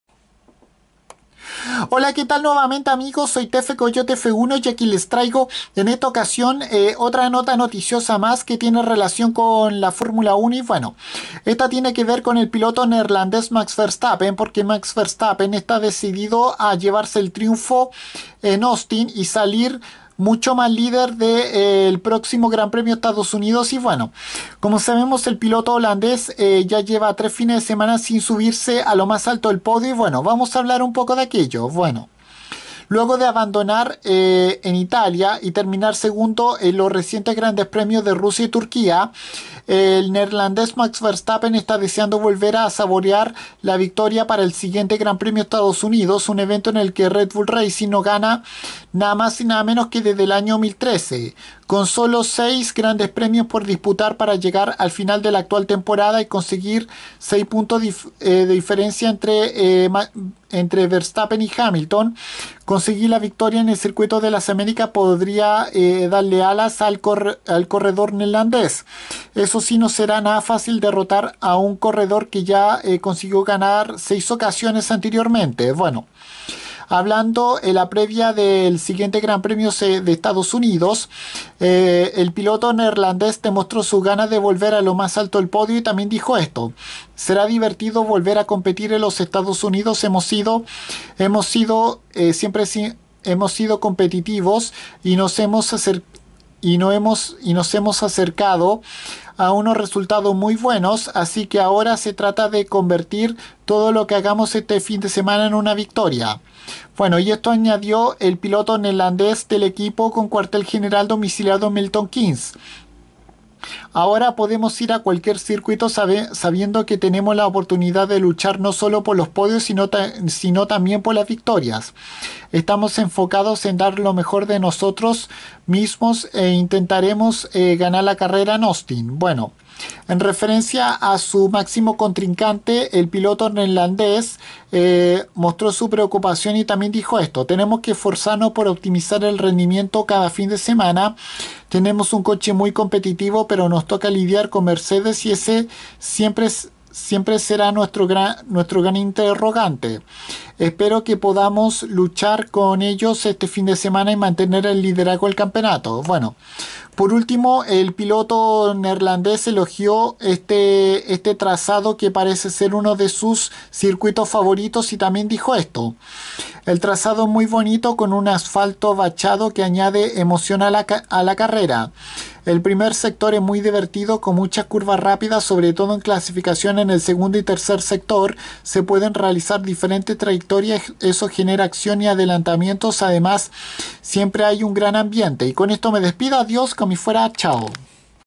Max Verstappen quiere ganar y salir mas líder todavía del próximo GP de Estados Unidos
Por ahora el piloto neerlandés solo lleva 3 Grandes Premios sin estar en los mas alto de podio